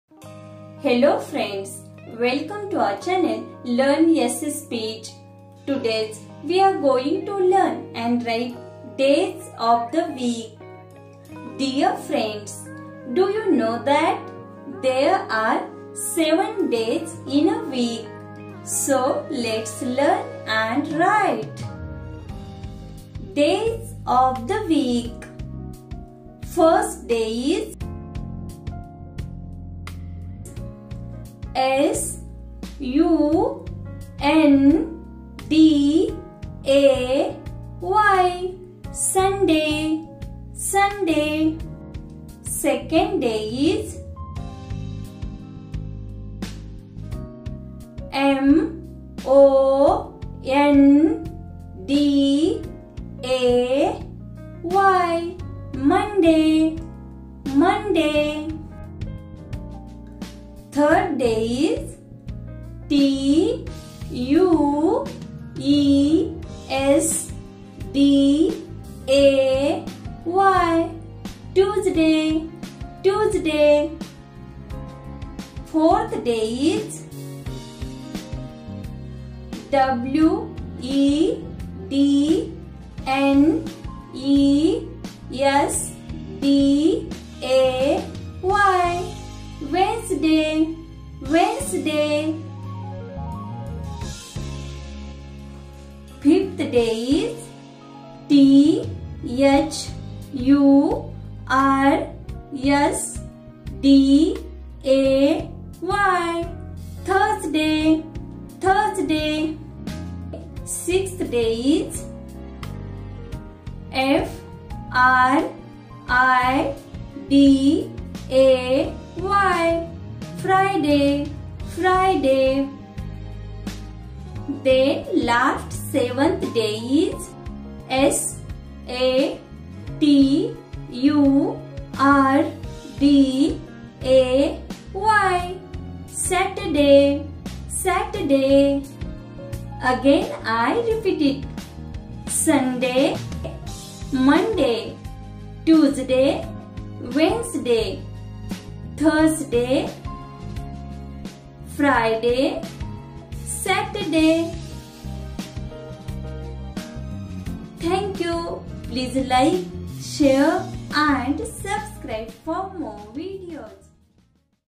Sunday Monday-Days of the week/Sunday Monday ki spelling/saptah ke dinon ke naam/sunday to saturday
Hello Friends,
In this video We will learn with my handwriting Days of the week with my best & easy way.
There are 7 days of week